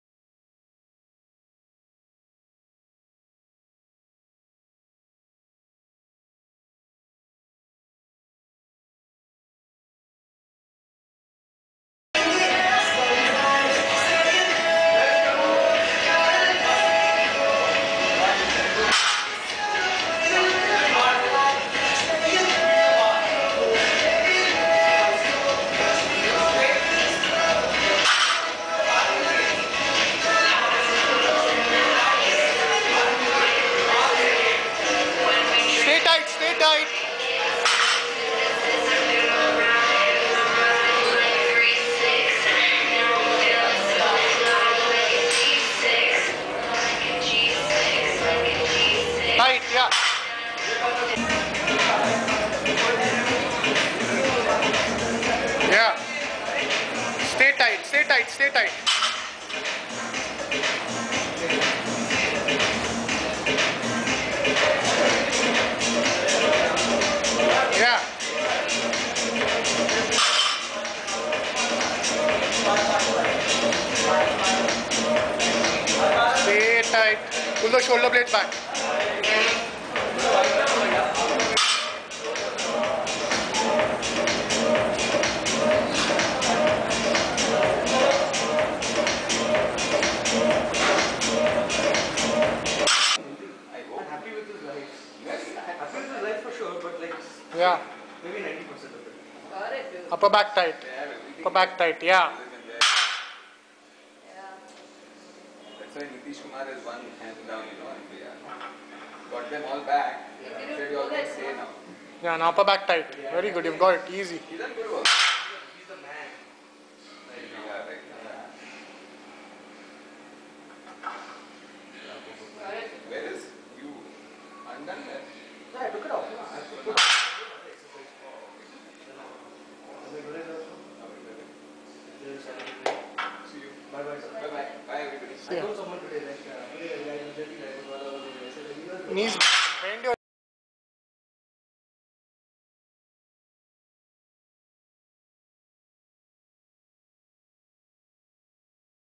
Bijoy DL 245x4.wmv
Deadlifts:
205 x 4
225 x 4
245 x 4